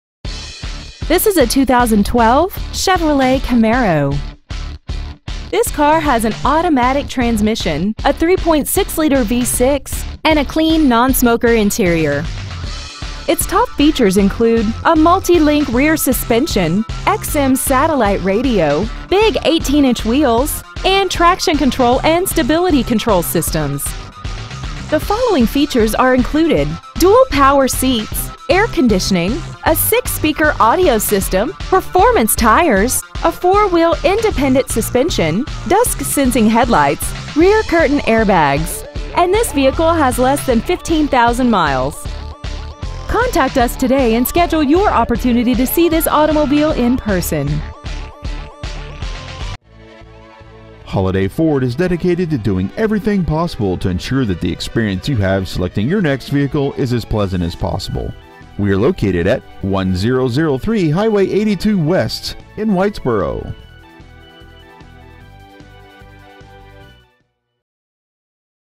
Preowned 2012 CHEVROLET CAMARO Whitesboro TX
Year : 2012
 Make : CHEVROLET
 Model : CAMARO
 Engine : 3.6L V6 24V GDI DOHC FLEXIBLE FUEL
 Trans . : AUTOMATIC
 Exterior : SUMMIT WHITE
 Miles : 14,133
 Interior : BLACK
 Stock : CP161476